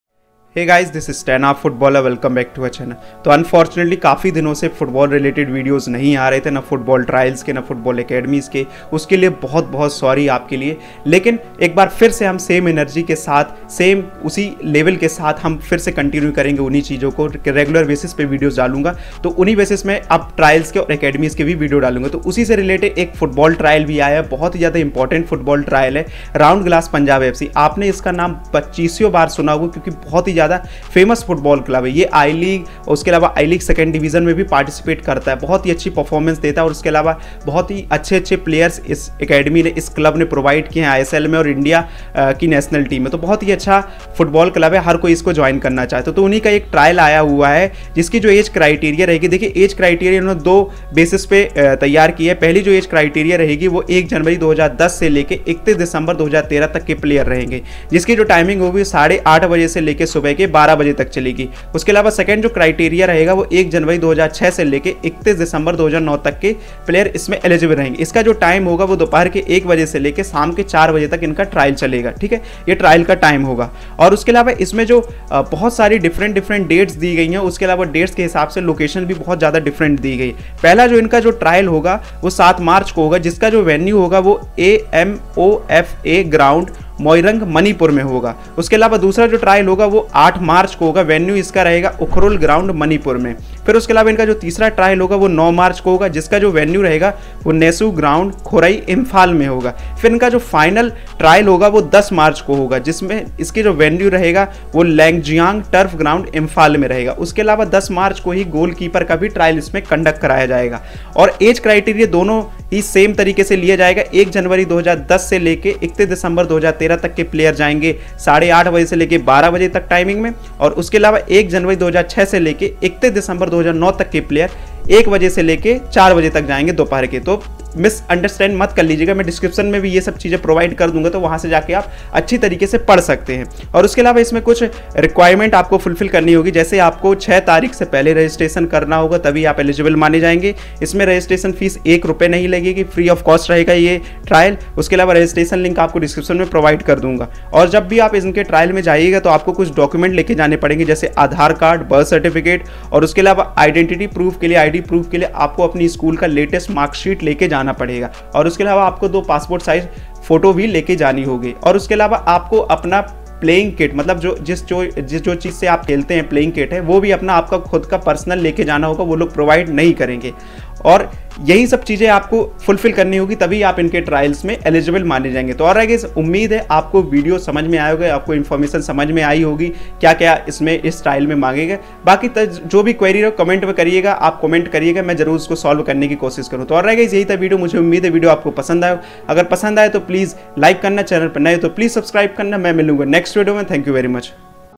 Round Glass Punjab FC Football trial | best football trial
Registration Deadline: 9:00 AM, 6th March 2022. Only selected candidates after registration will be invited for trials.

Note: Only selected candidates after registration will be invited for trials.

7th March, AMOFA Ground, Moirang
DOB – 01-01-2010 to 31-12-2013, 8:30 AM – 12:30 PM
DOB – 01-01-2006 to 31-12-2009, 1:30 PM – 4:30 PM

8th March, Ukhrul Ground
DOB – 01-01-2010 to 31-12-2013, 9:00 AM – 12:00 PM
DOB – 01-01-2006 to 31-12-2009, 1:00 PM – 4:00 PM

9th March, Nesu Ground, Khurai, Imphal

10th March, Langjing Turf Ground, Imphal (Goalkeeper and Final Trials)
Goalkeeper Trials – DOB – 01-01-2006 to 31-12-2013, 8:30 AM – 12:30 PM
Final Round – DOB – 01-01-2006 to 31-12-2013, 1:30 PM – 4:30 PM

1. Only selected candidates after registration will be invited for trials.
2. Every player needs to arrive 30 minutes prior to the trial starting time.
3. Every player needs to carry his training kit, football shoes, water bottle, snacks and any other personal items.
4. Every player is required to carry Aadhar card, birth certificate and latest school marksheet for ID proof.
5. The club and its partner organization will not be responsible for any kind of injury happened during the trials.
6. Decision of the scout/coach will be final under all circumstances.
7. These trials will be free of cost.